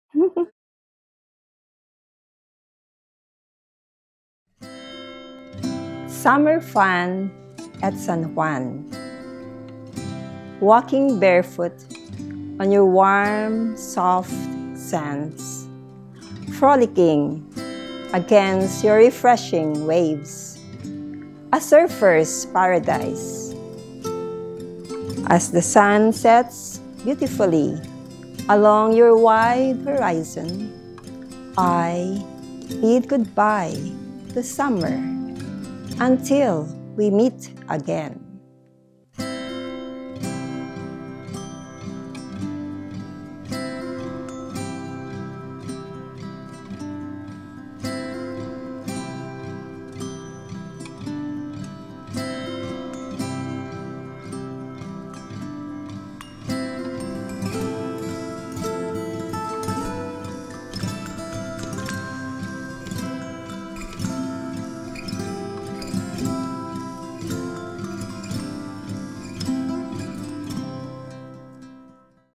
Do you also miss the Beach and the Sea ? | Summer Fun at San Juan | Cure Quotes with CynthMD
Thank you to summer and welcome to the next season, knowing that every new chapter brings a treasure of its own.
Listen and may you find healing in your hearts.

Special participation:
Our sweet pet cat, Yassir, who freely decided to join us in the shoot.

Join us every Sunday at 8am (Philippine time) for a new CureQuote. We hope it touches your life in a meaningful way.